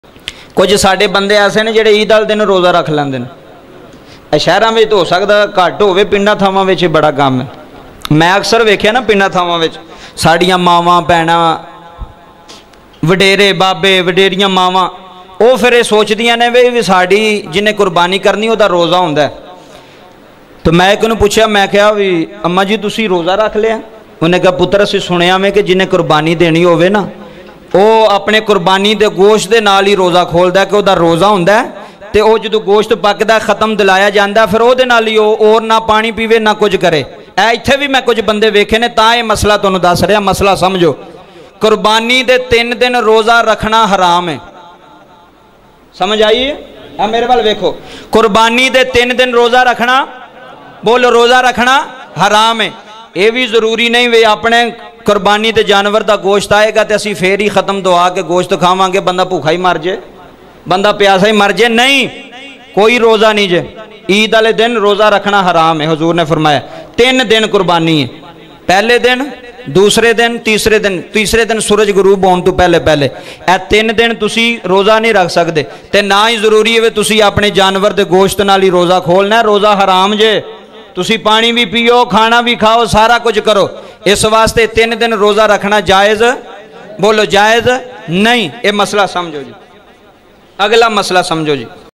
Eid Ke Din Roza Rakhna Kaisa Hai ?// Allama Zahid Nadeem Sultani //kaleji Roza#Wajdan_Sound_Sialkot
Title =  Eid Ke Din Roza Rakhna Kaisa Hai ?
Voice =  Allama Zahid Nadeem Sultani